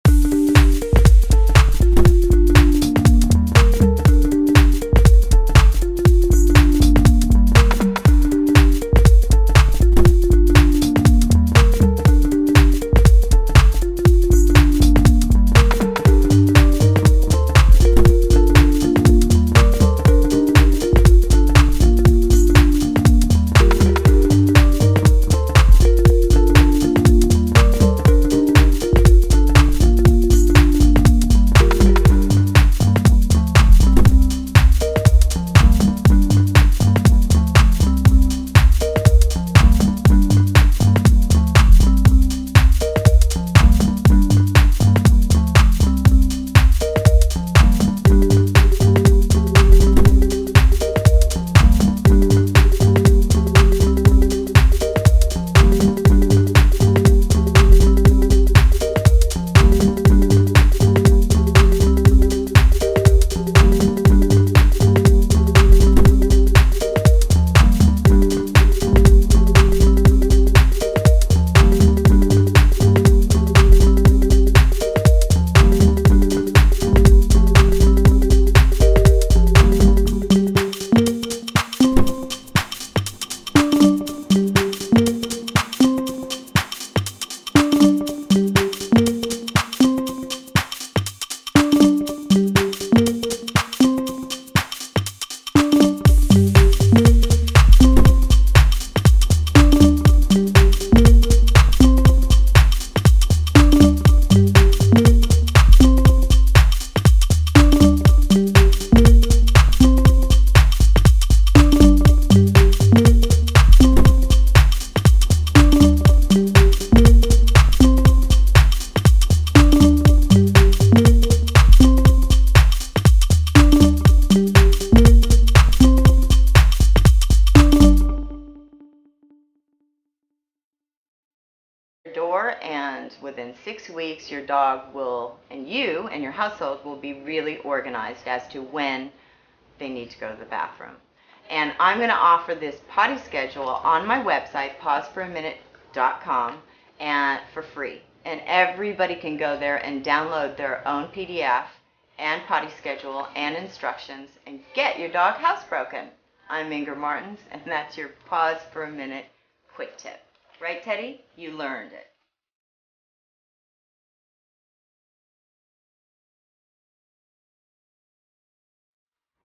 How To Housetrain Your Dog | Perez Hilton
TeddyHilton.com special correspondent, celebrity dog trainer Inger Martens, gives you some helpful tips on potty training your pooches!

Check it out (above)!!